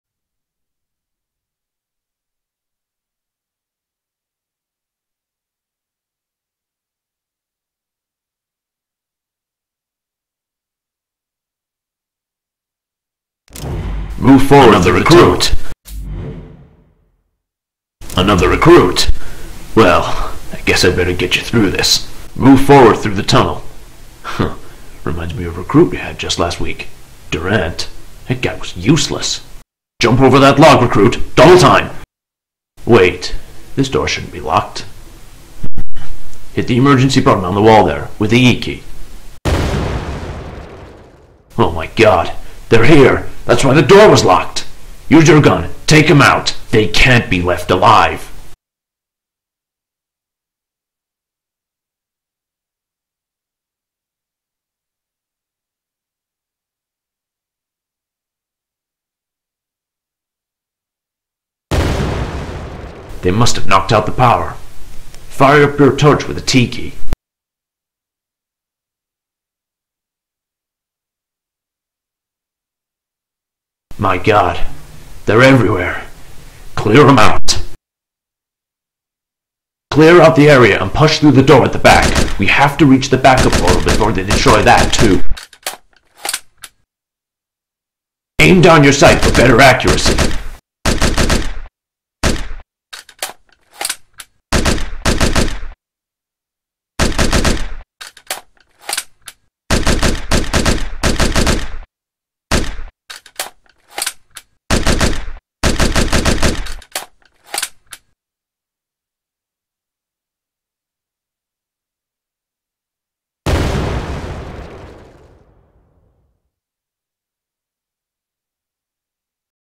Zombie Game Development Log: Tutorial Level Test
A quick test of the tutorial level within my zombie game.